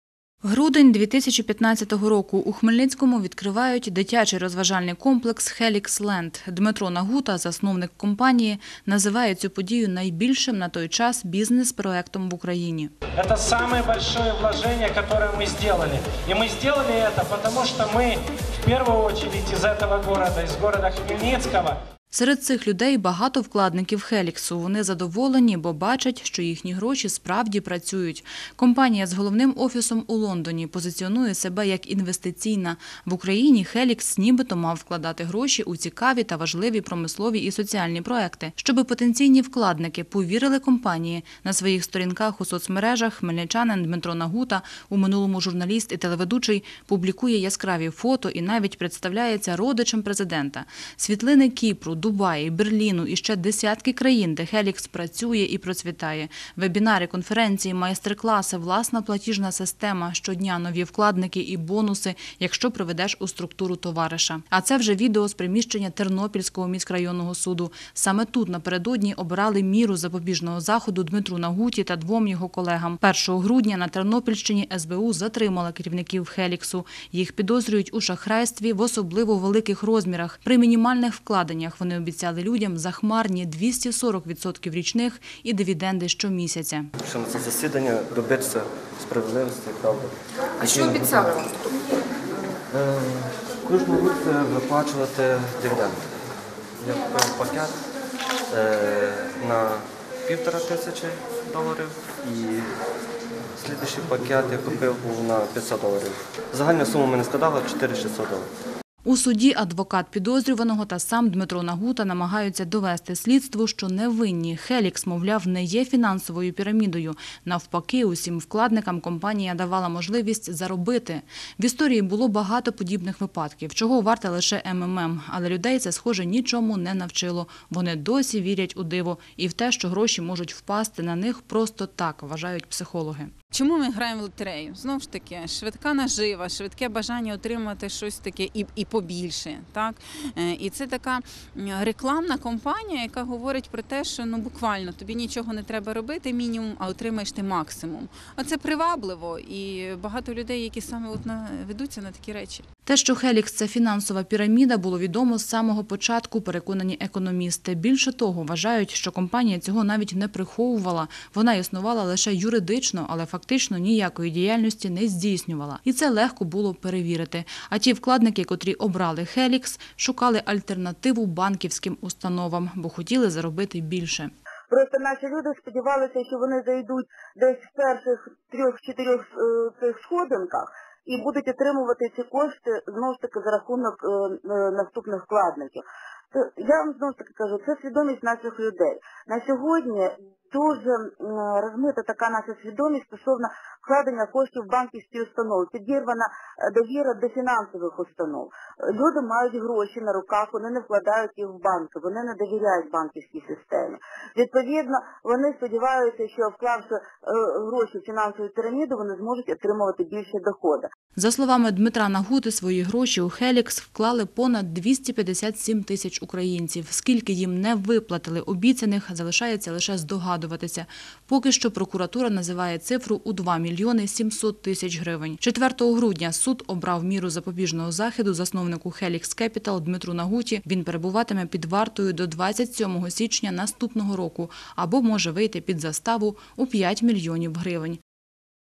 Афера на мільйони. Засновників «Helix» підозрюють у шахрайстві
Інвестиційна компанія чи фінансова піраміда? 1 грудня засновника компанії «Хелікс» хмельничанина Дмитра Нагуту і двох його колег затримали працівники СБУ. Це сталося у сусідній Тернопільській області. Їх підозрюють в організації фінансової піраміди і у шахрайстві в особливо великих розмірах. За повідомленням прокуратури, своїх вкладників компанія надурила майже на 3 мільйони гривень.